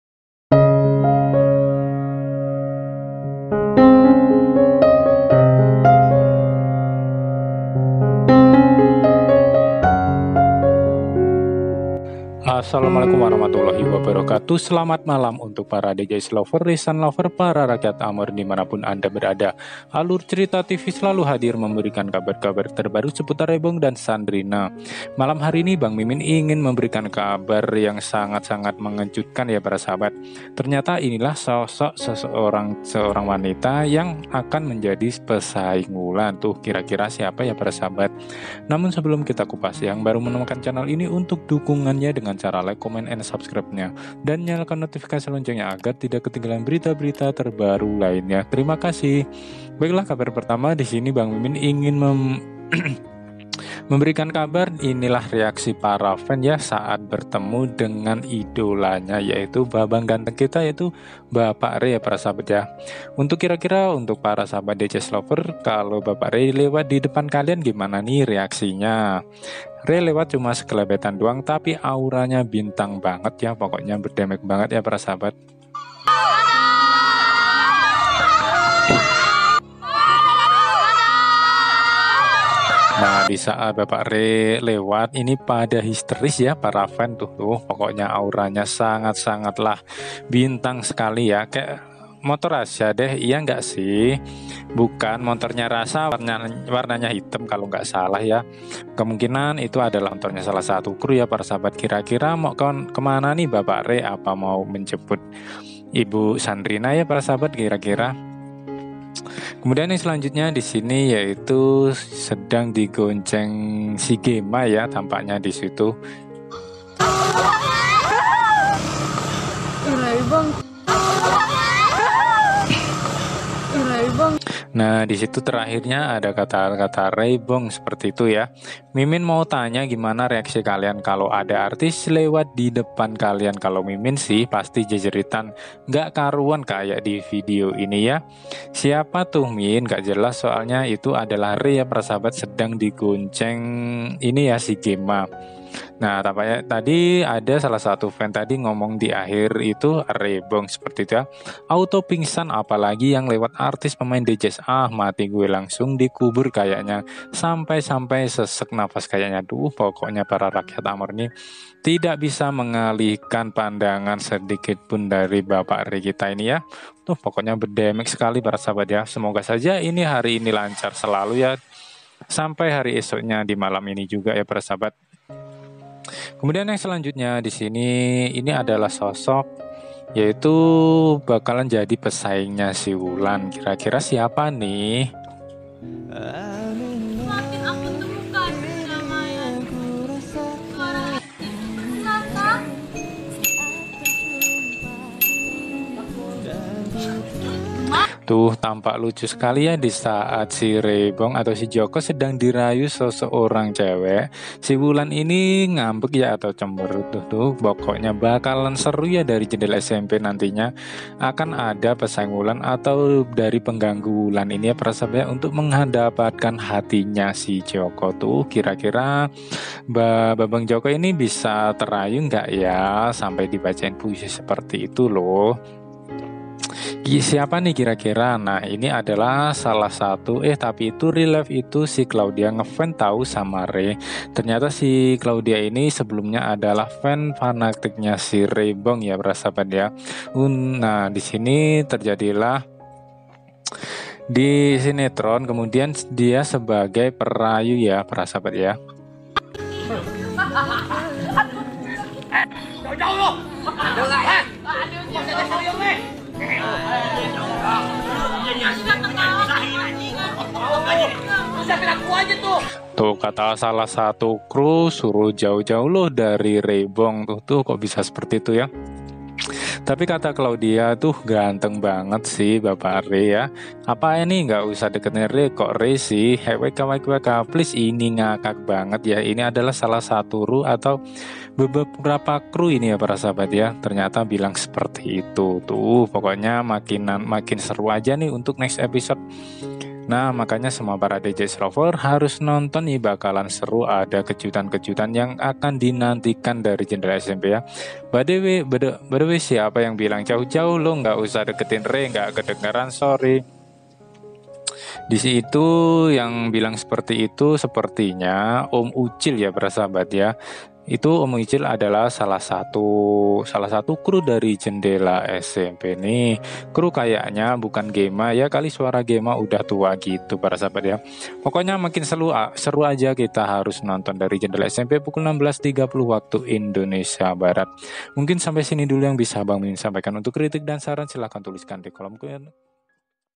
Inilah Sosok Yg Bakal Menjadi Pesaing Wulan Dlm Memperebutkan Hati Joko Di Sinetron Dari Jendela SMP
_Nama Artis muda Rey bong dan sandrinna michelle Kian Melambung tinggi  Setelah Mereka berdua Membintangi dan sekaligus Dipasangkan dalam sinetron dari jendela smp SCTV.

Banyak sekali Fans Reysan Yang berharap Agar Rey bong dan Sandrinna michelle berjodoh Didunia nyata.Banayak fans yg mengaku baper ( Bawa perasaan ) Setelah Melihat Cemistri keduanya di sinetron Dari jendela smp Hingga akhir nya mereka berduapun Dido'akan berjodoh.

Karna Di chanel ini kalian Akan Slalu mendapatkan Kabar terbaru Seputar Rey bong dan Sandrinna michelle.
__SEKIAN & TERIMAKASIH___